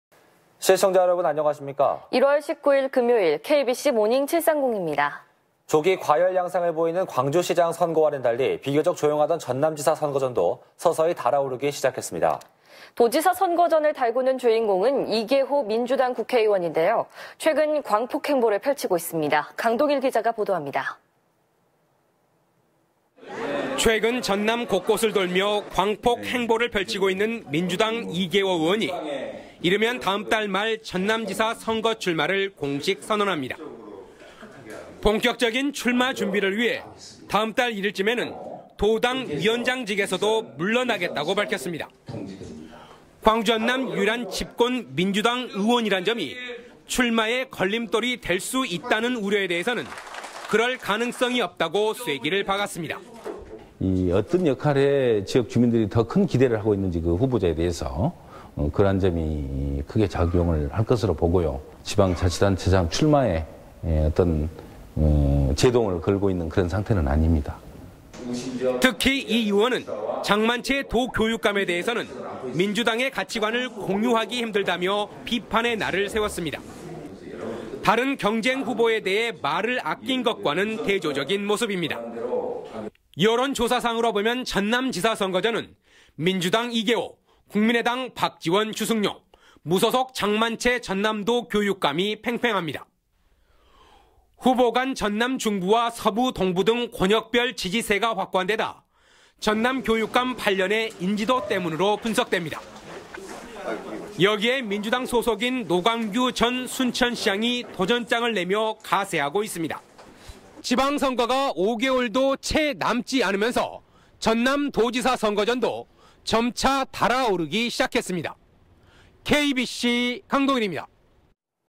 이개호, 전남지사 출마 굳혔다..광폭 행보
조기 과열 양상을 보이는 광주시장 선거와는 달리 비교적 조용하던 전남지사 선거전도 서서히 달아오르기 시작했습니다.

도지사 선거전을 달구는 주인공은 이개호 민주당 국회의원인데요 .최근 광폭 행보를 펼치고 있습니다.

강동일기자가 보도합니다.